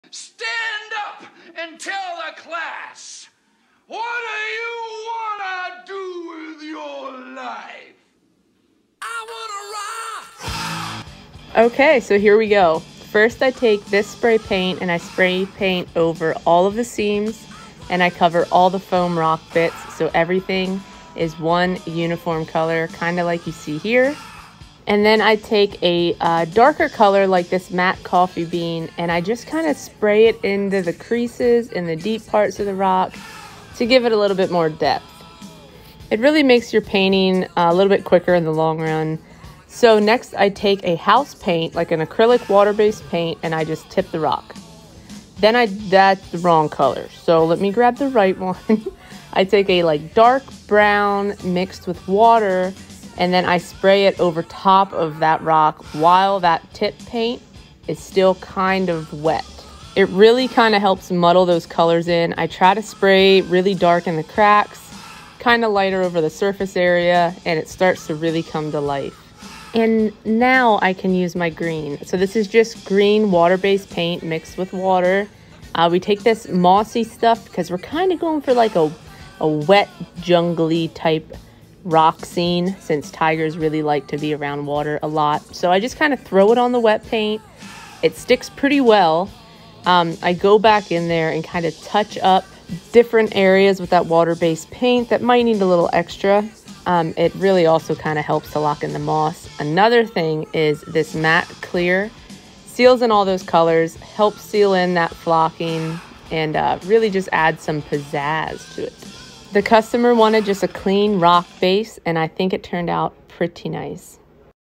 How to paint a realistic habitat rock
Learn how I paint a gray rock with realistic moss/algae.
Matte Vintage Gray spray paint
Matte Coffee Bean spray paint
Light grayish green interior house paint/acrylic latex
Dark umber water based polytranspar
Light green water based poly transpar
Rimbak artificial moss and lichen
Matter clear spray paint